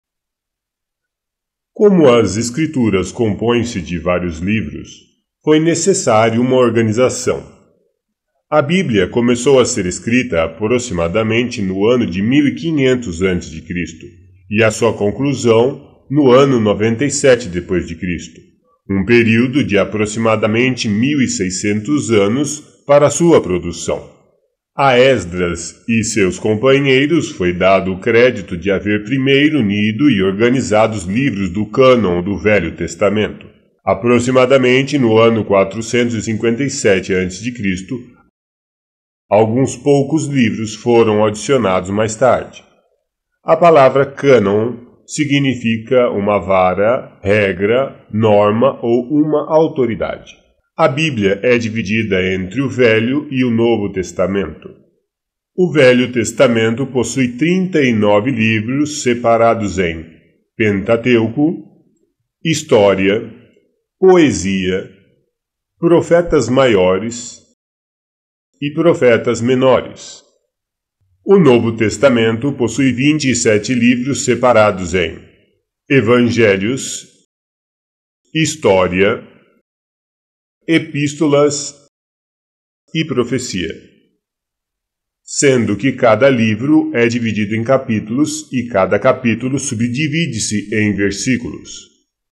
Divisões da Bíblia (temporário) -P
Como as Escrituras compõem-se de vários livros, foi necessário sua organização.
A Bíblia começou a ser escrita aproximadamente no ano de 1500 A.C e a sua conclusão no ano 97 d.C um período de aproximadamente 1600 anos para sua produção.
A Esdras e seus companheiros foi dado o crédito de haver primeiro unido e organizado os livros do cânon do Velho Testamento. aproximadamente no ano 457 A.C, alguns poucos livros foram adicionados mais tarde
A palavra cânon significa uma vara, regra, norma ou uma autoridade.
A Bíblia é dividida entre o Velho e Novo Testamento.
O Velho Testamento possui trinta e nove livros separados em:
Pentateuco - Gênesis, Êxodo, Levítico, Números, Deuteronômio.
Poesia - Jó, Salmos, Provérbios, Eclesiastes, Cantares de Salomão
Profetas Maiores - Isaías, Jeremias, Lamentações, Ezequiel, Daniel
Profetas Menores - Oséias, Joel, Amós, Obadias, Jonas, Miquéias, Naum, Habacuque, Sofonias, Ageu, Zacarias, Malaquias.
O Novo Testamento possui vinte e sete livros separados em:
Evangelhos - Mateus, Marcos, Lucas, João.
História - Atos dos Apóstolos.
Epístolas - Romanos, IeII Coríntios, Gálatas, Efésios, Filipenses, Colossenses, IeII Tessalonicenses, IeII Timóteo, Tito, Filemom, Hebreus, Tiago, IeII Pedro, I,IIeII João, Judas.
Profecia - O Livro do Apocalipse.
Sendo que cada livro é dividido em capítulos e cada capítulo subdivide-se em versículos.